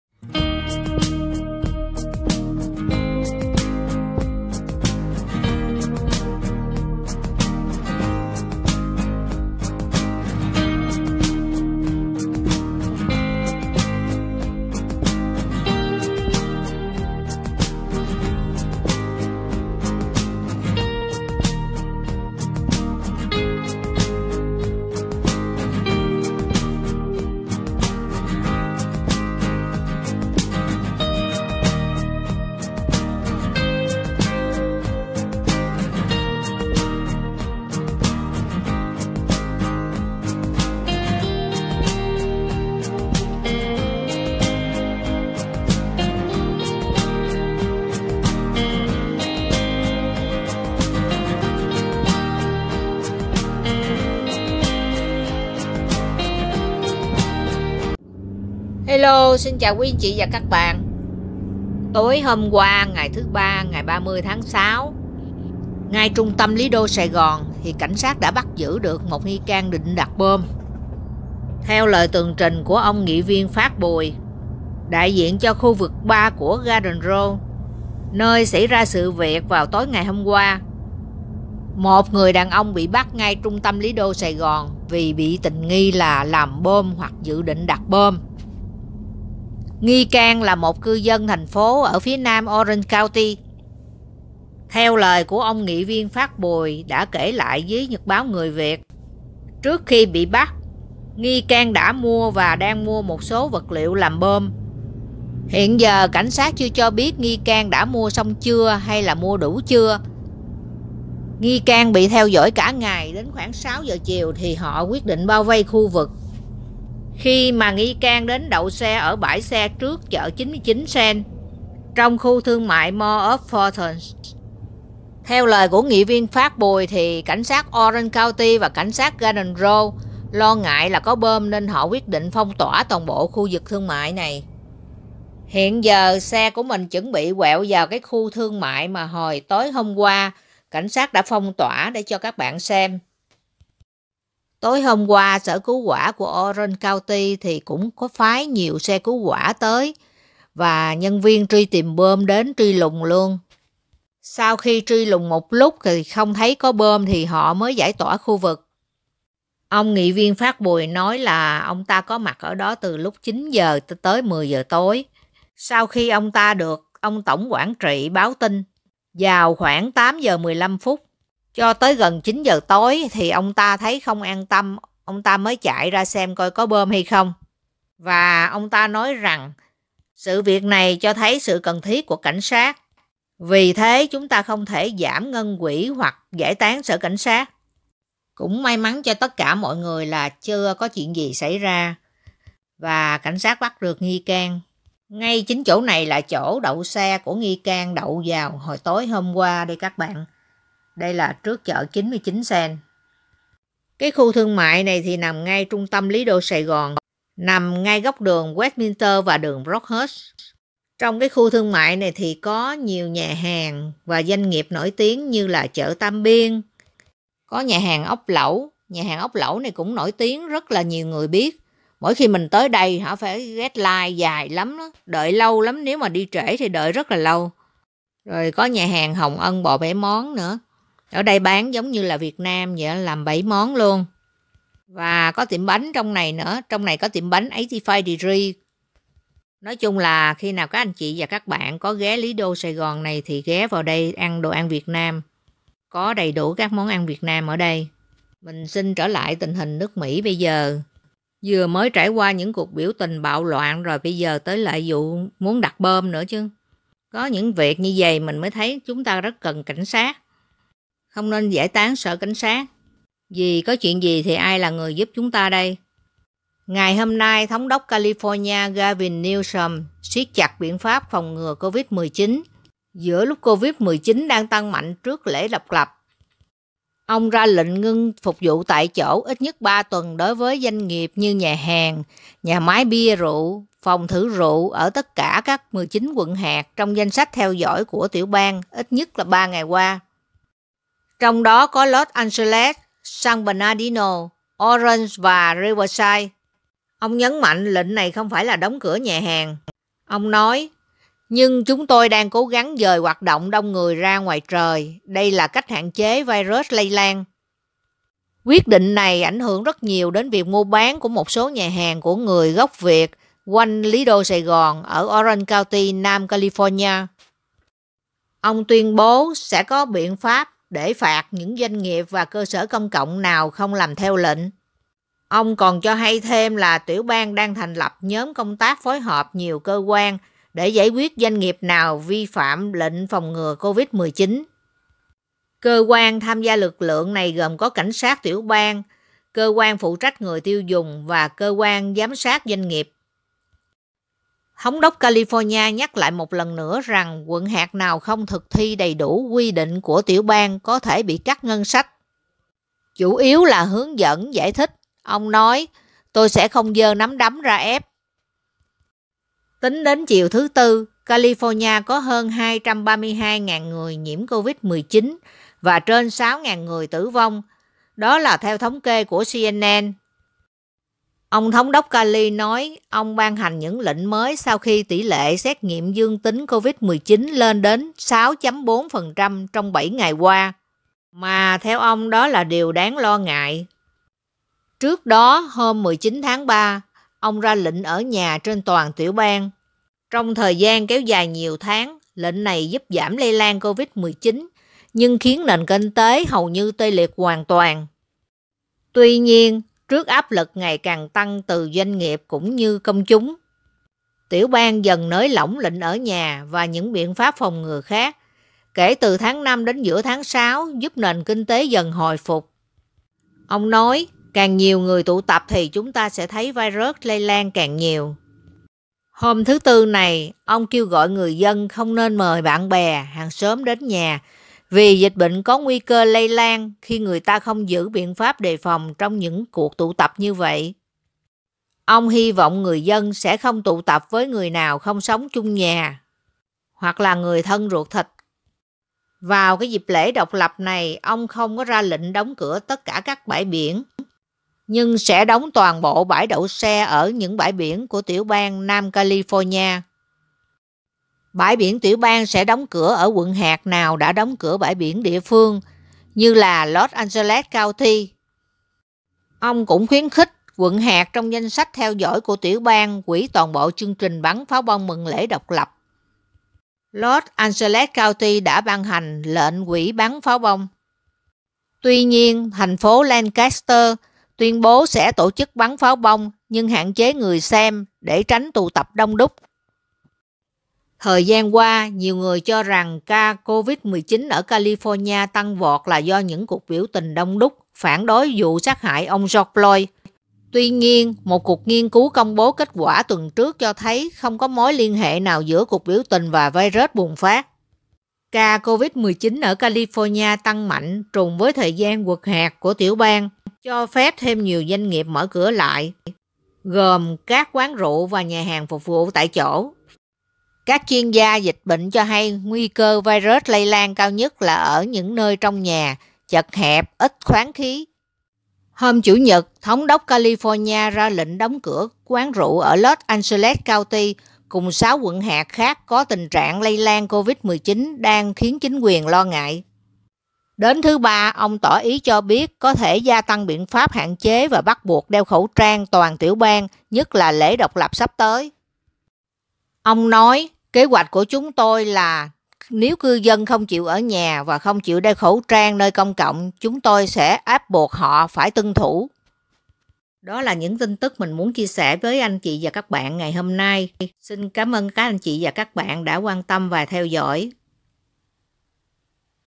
Cuộc sống ở Mỹ. Tình hình ở Mỹ. California con số tăng kỹ lục trong một ngày.
Cuộc sống ở Mỹ. Tình hình ở Mỹ. California số ca tăng kỹ lục .Thống đốc California đã ra lệnh xiết chặt phòng ngừa.